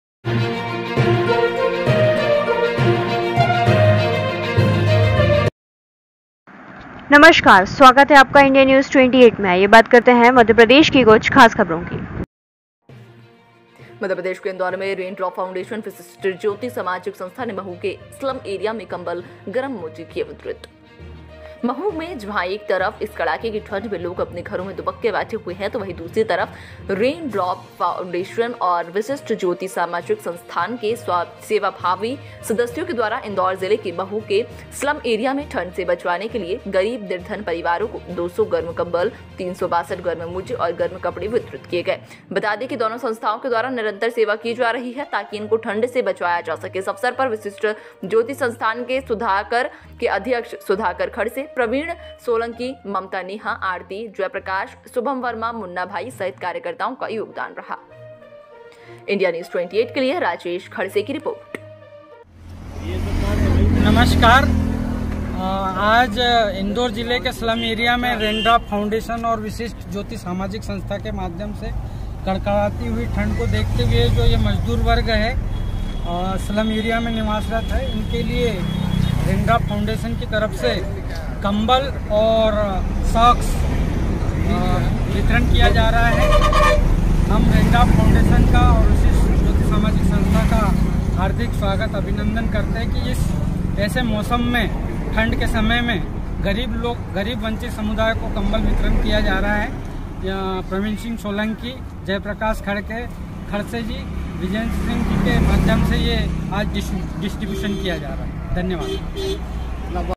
इंदौर में विशिष्ट ज्योति सामाजिक संस्थान ने महू के स्लम एरिया में कम्बल, गरम मौजे वितरित किये
Official